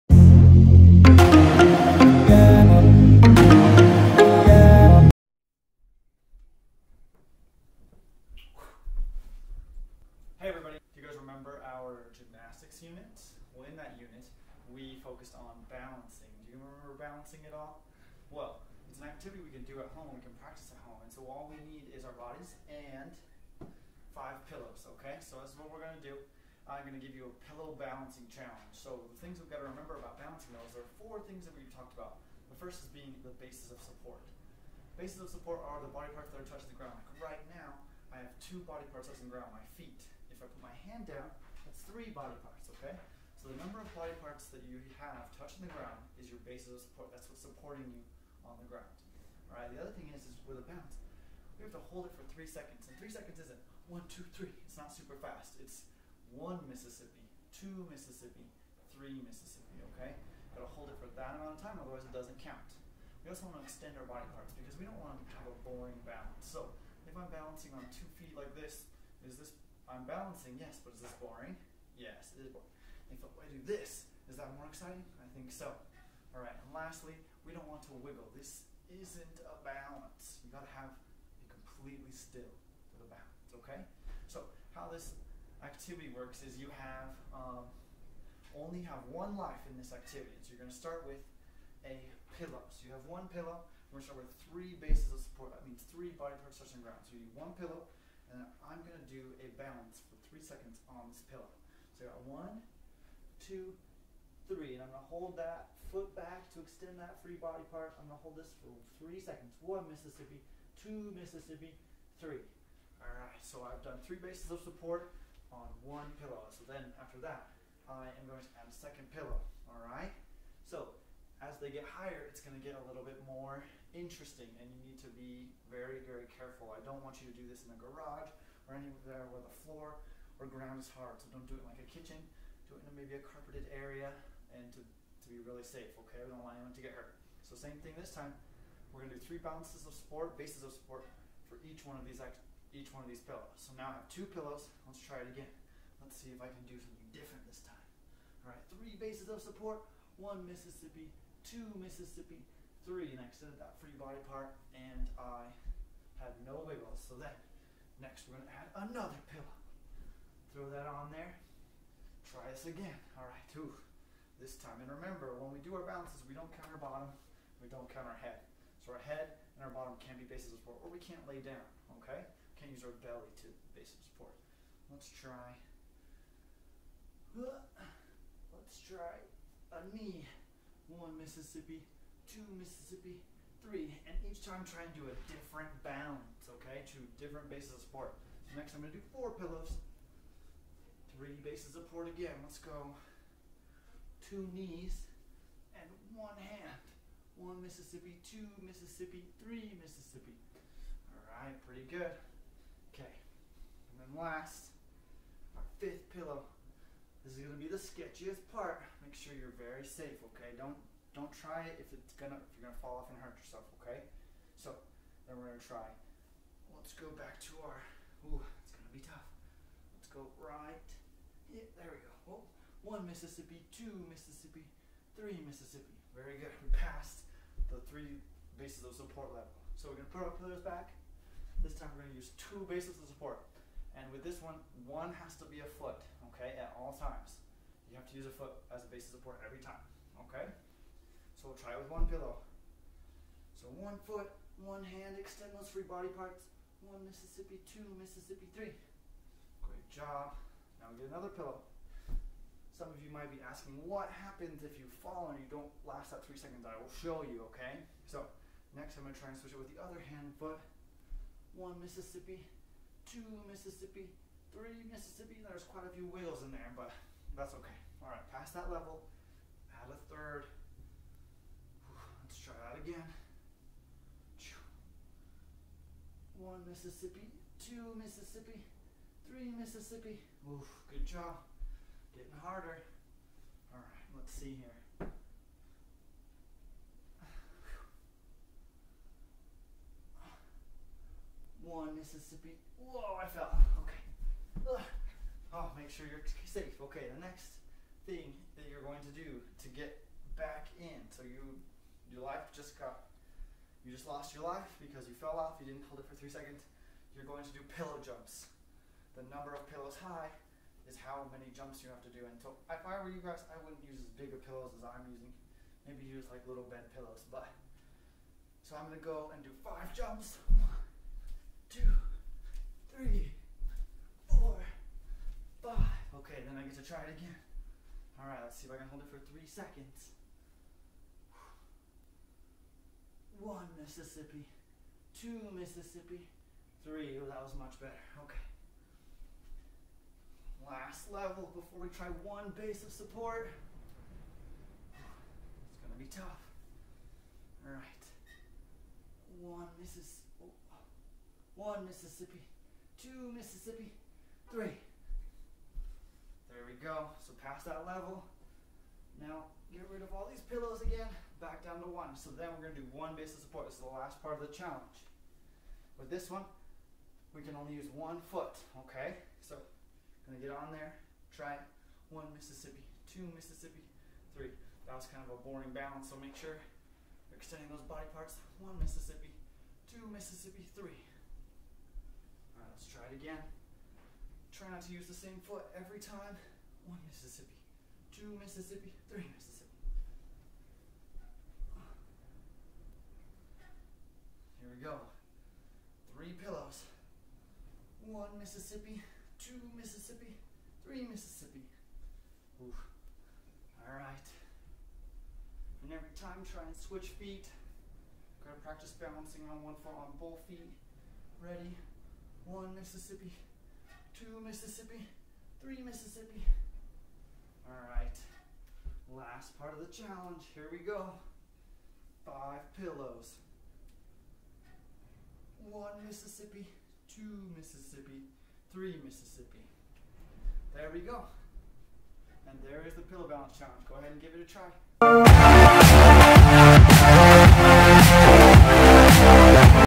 Pillow Balancing
Remember our Gymnastics Unit?
Part of that unit focused on balancing, which you can do anywhere. So here is a challenge to do at home with your body and some pillows (5 to be exact).
When it comes to balancing we need to remember the four skill cues.
1. Bases of support (the body parts touching the ground).
2. Hold for three seconds (Mississippi's).
3. Extend free body parts.
4. No wiggles.

First and foremost, be safe! Pick a place that if you fall it isn't going to hurt and don't try something if you don't feel comfortable.